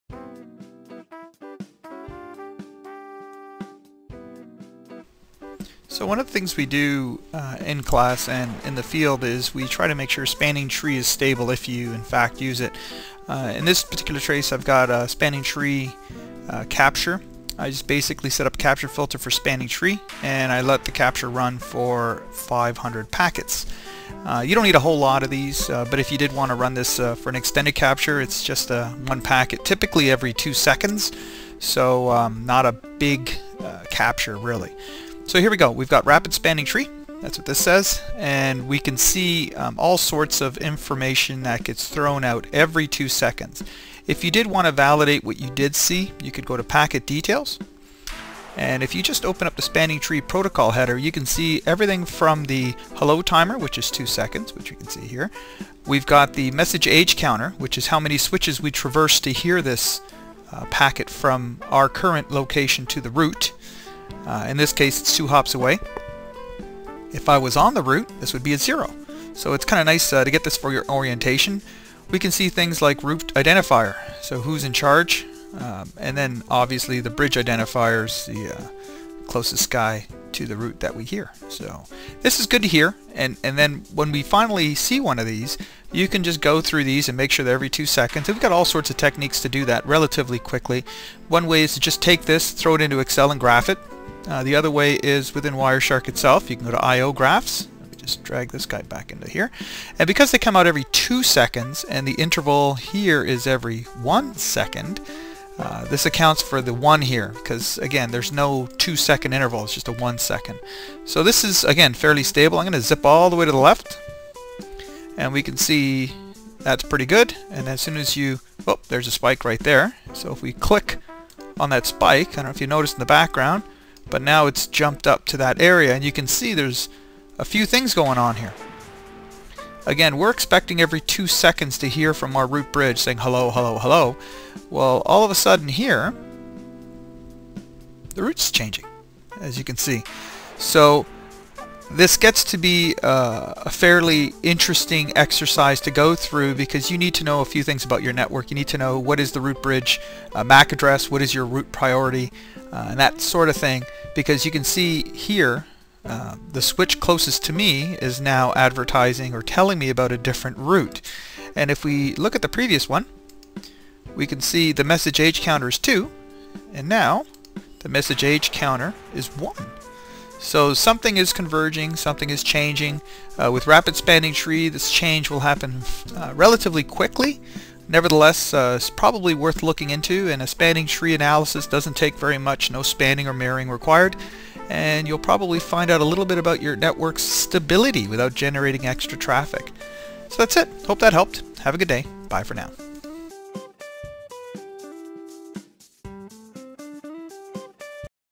Analyzing Spanning Tree With Wireshark
In this video I explain some of the basic items we look for when we capture some Spanning Tree packets.